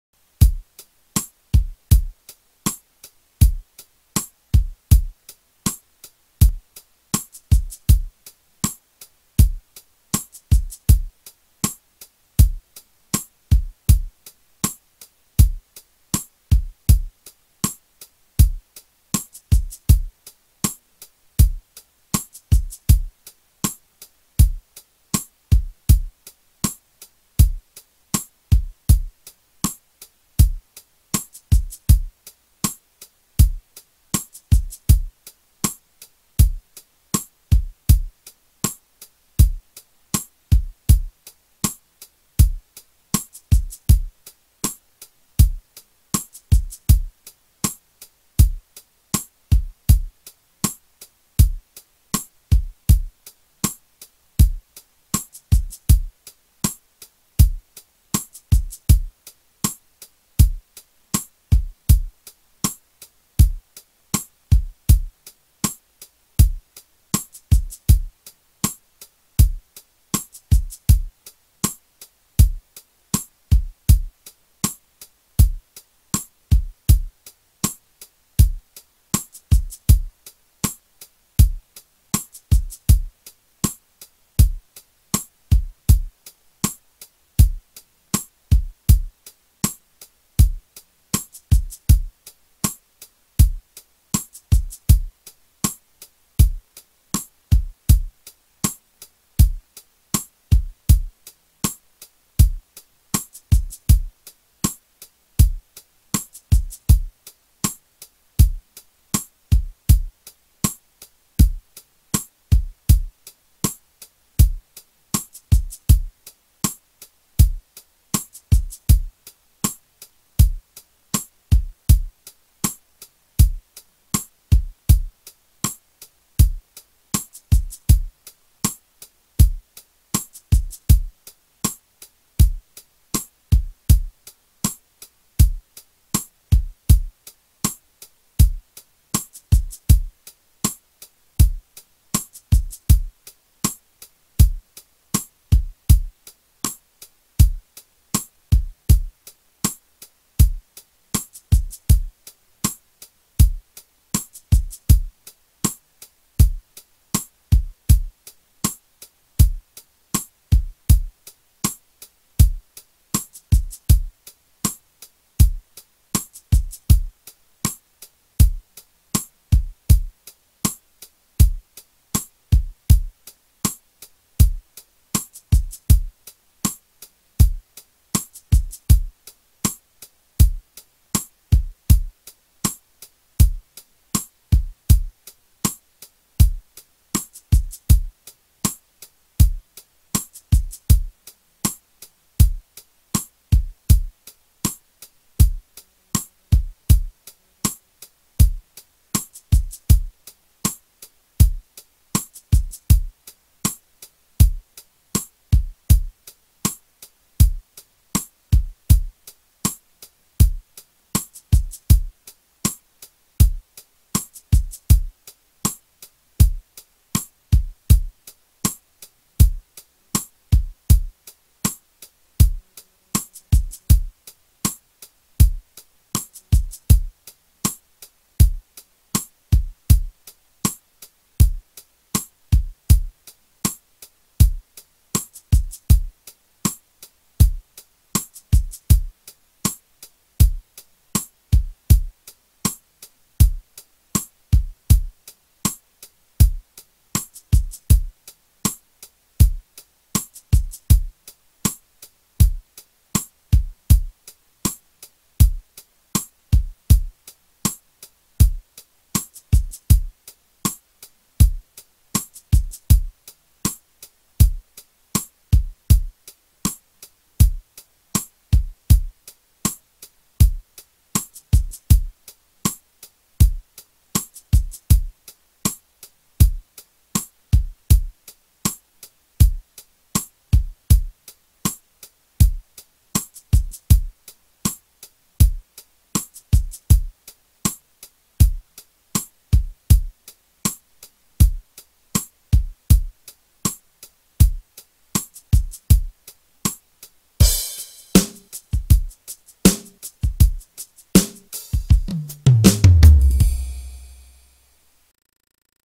BASE BALADA SUAVE 90 BPM PRÁCTICA| APRENDE PIANO DE OÍDO| 04
►¿Quieres aprender piano?  Prueba este efectivo método

 Este es el CUARTO video de una serie que te permitirá tocar y acompañarte con esta base de percusión. Te dejo el ritmo musical BALADA SUAVE a 90 BPM. Espero que lo disfrutes. Seguiré subiendo más ritmos en cuanto pueda.

La idea de entregarte estas bases de percusión es para que puedas practicar con tu instrumento. Pones a sonar la pista de la base, te imaginas que alguien la está tocando y después d e1.2.3.y 4...inicias a tocar. Esto te ayudará a mejorar tu habilidad para tocar en grupos musicales, además te ayudará a llevar bien el tiempo o tempo de la canción. Saca el mejor provecho de estas herramientas que te estoy entregando. Si tienes dudas o preguntas solo tienes que avisarme y te respondo en la medida de lo posible. GRACIAS POR SEGUIR NUESTRO CONTENIDO.

¡NO PIERDAS MÁS TIEMPO, VEN Y MIRA...!